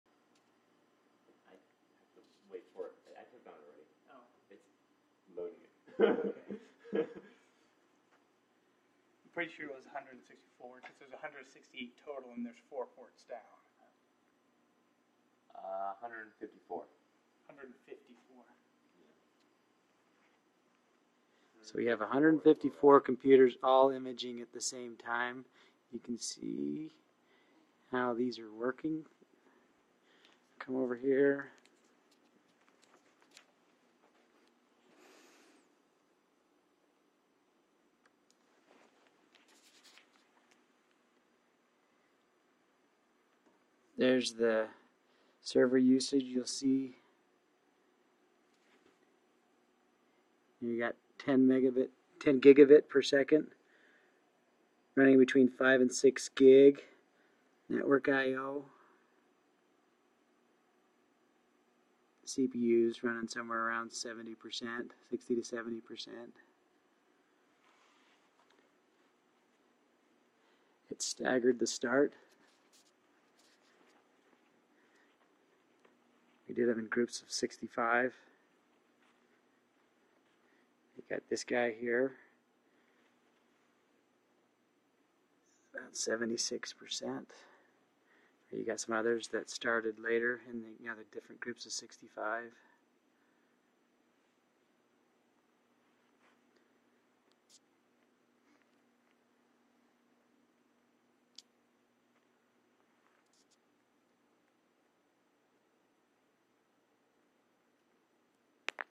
Imaging 154 HP Thin clients in the SLCC Math Emporium Lab
After getting everything powered on and connected to the network, we pushed an updated image to the new HP T5470e Thin Clients in the open area of this new lab in about 28 minutes.  The Disk image is 4 Gig on the servers hard drive but really the WES7 image is about 2 Gig.  The virtual server (running HP Device manager) was running around 5-6 Gbps network IO, with 70% CPU utilization.  The clients each pull the image via FTP from the server.

If you think of that in terms of amount of time per device that ends up around 11 seconds each to image.